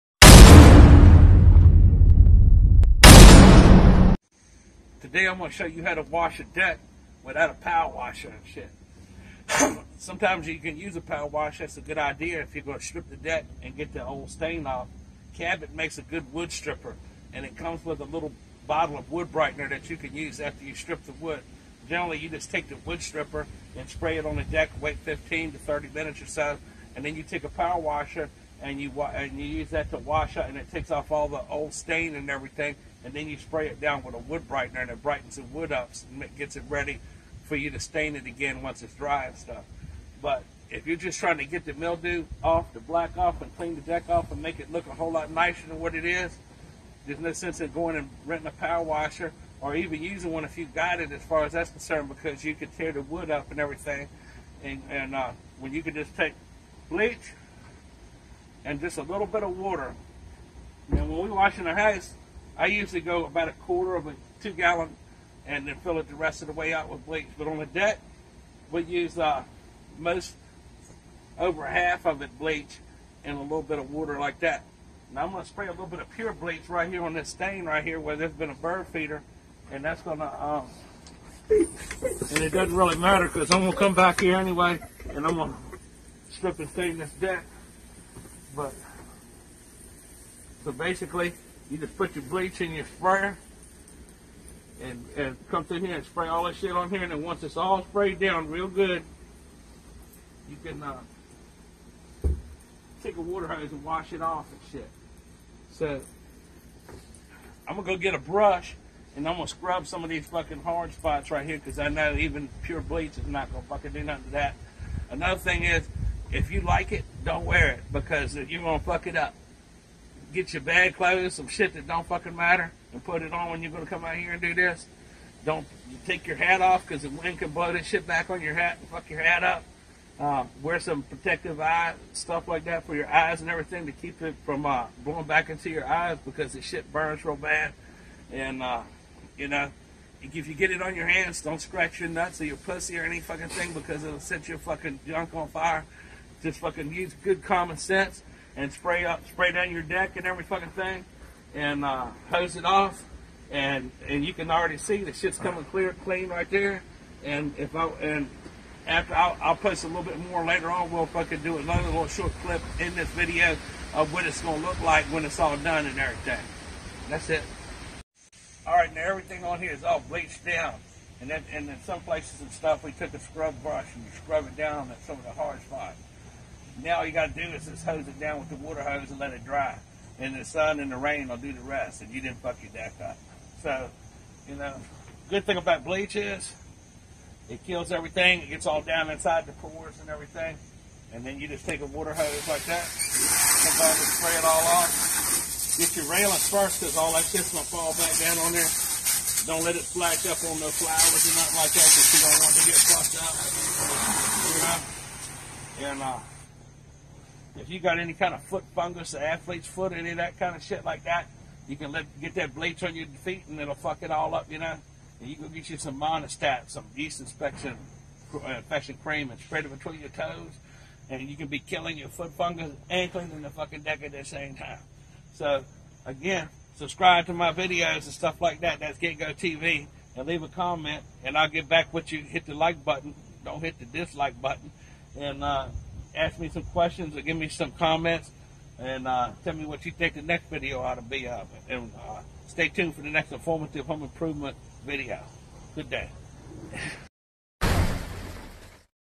HOW TO WASH YOUR DECK WITHOUT A POWER WASHER
In this video you will learn how to wash your deck without a power washer which can damage your wood and cause it to splinter. I will show you a simple easy inexpensive method that I use to make old Dirty decks look clean and new again without any scrubbing or power washing